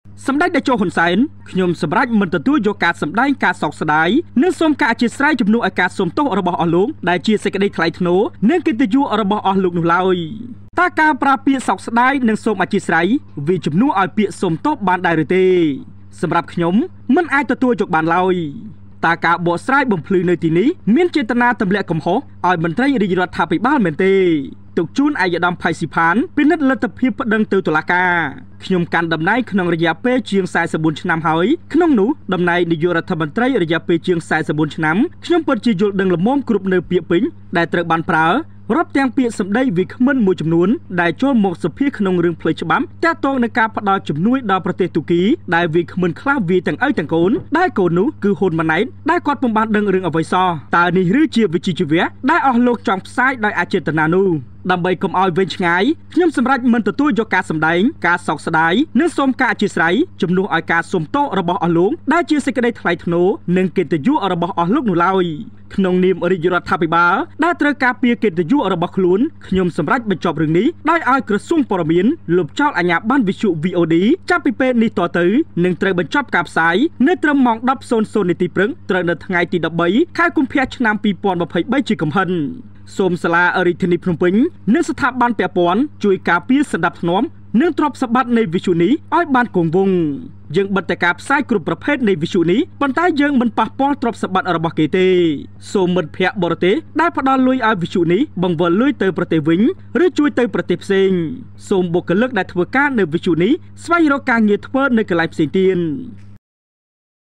Khmer News, សម្តេចតេជោ ហ៊ុន សែន ៖ ខ្ញុំសម្រេចមិន ទទួលយកការសំដែងការសោកស្តាយនិងសូមការអធ្យាស្រ័យជំនួស
សម្តេចតេជោ ហ៊ុន សែន ៖ ខ្ញុំសម្រេចមិន ទទួលយកការសំដែងការសោកស្តាយនិងសូមការអធ្យាស្រ័យជំនួសឲ្យការសុំទោស
របស់អស់លោកដែលជាសេចក្តីថ្លៃថ្នូរនិងកិត្តិយសរបស់អស់លោកនោះឡើយ!